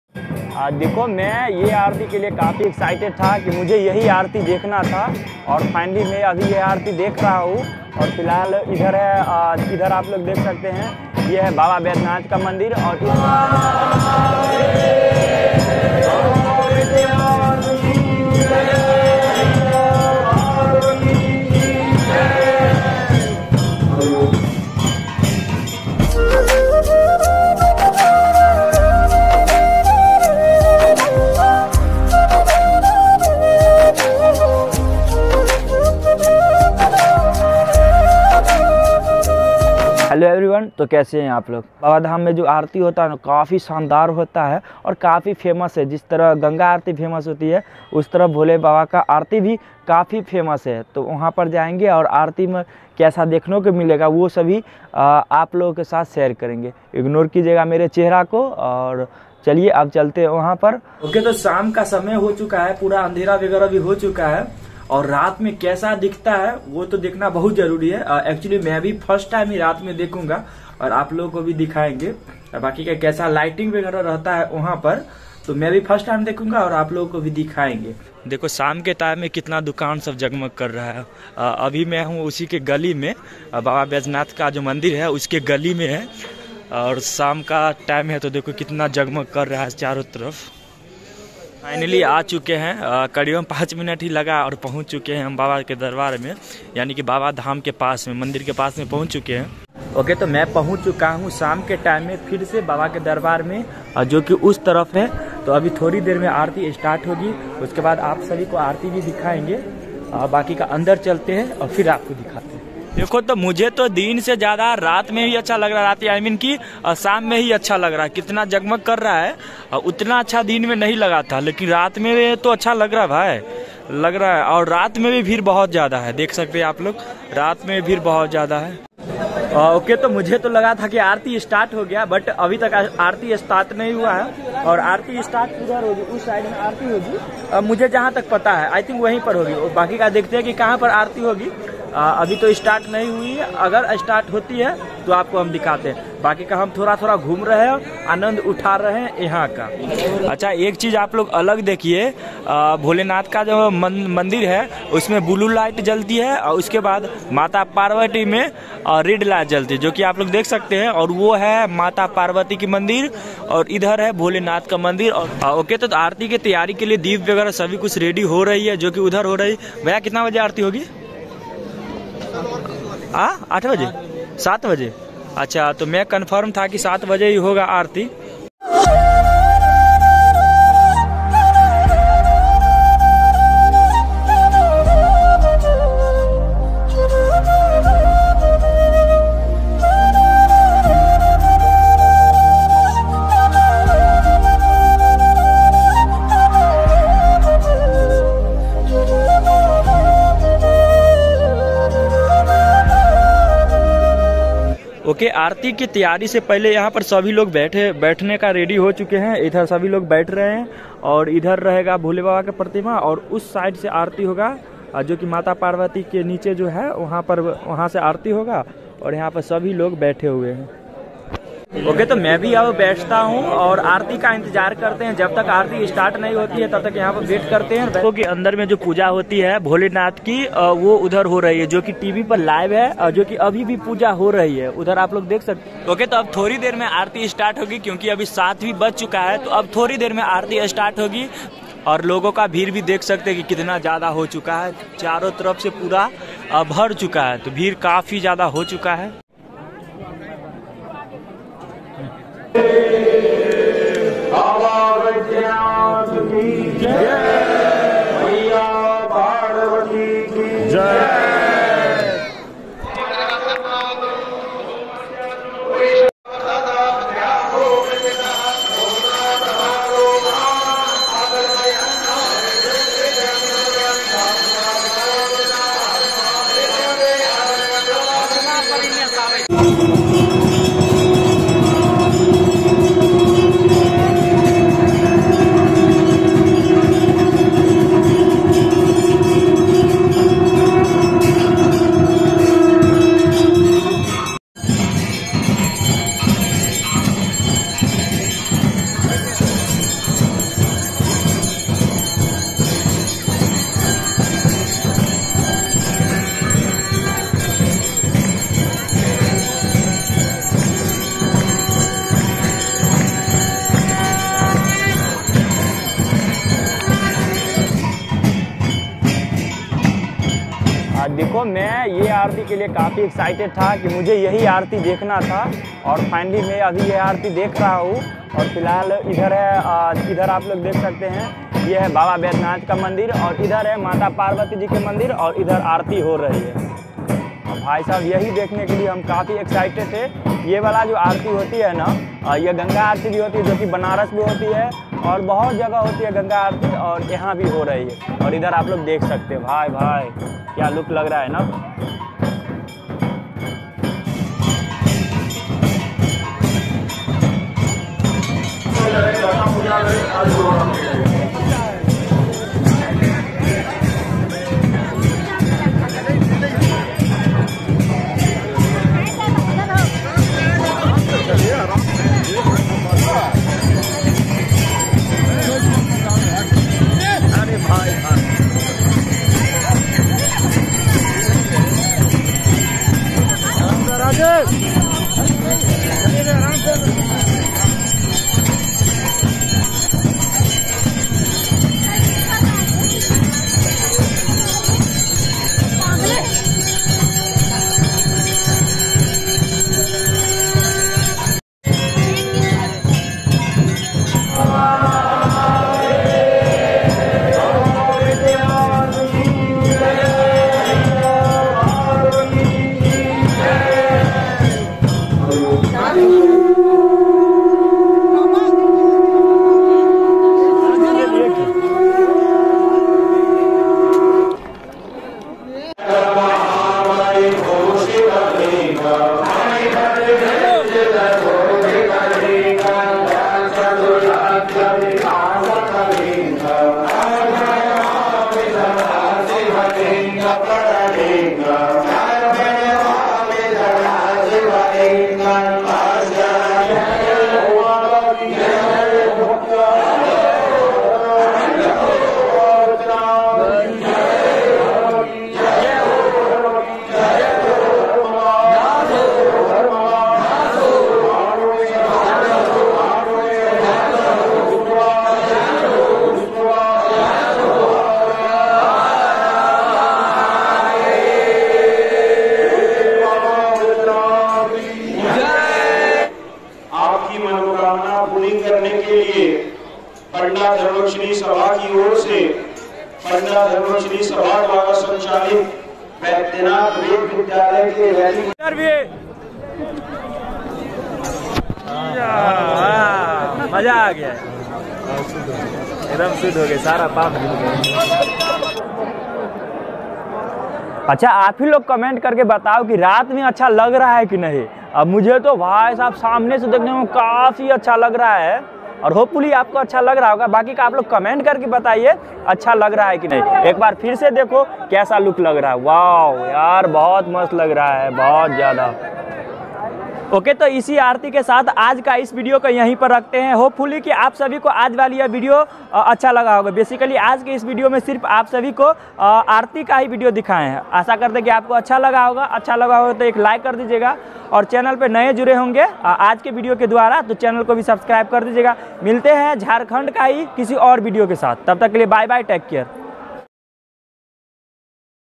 संध्या आरती देवघर 🙏Baba Baidyanath Dham Deoghar|Baba Dham Jharkhand|Devghar
भारत का प्रसिद्ध संध्या आरती 🙏Baba Baidyanath Dham Deoghar|Baba Dham Jharkhand|

hello doston aaj ki is video mein aap sabhi ko Baba Dham yani ki dip ghar mein jo Sandhya Aarti hoti hai uska video dikhayen Hain yah sandesh Aarti pure Bharat mein bahut famous hai to baki ka aap log jaaiye video dekhiae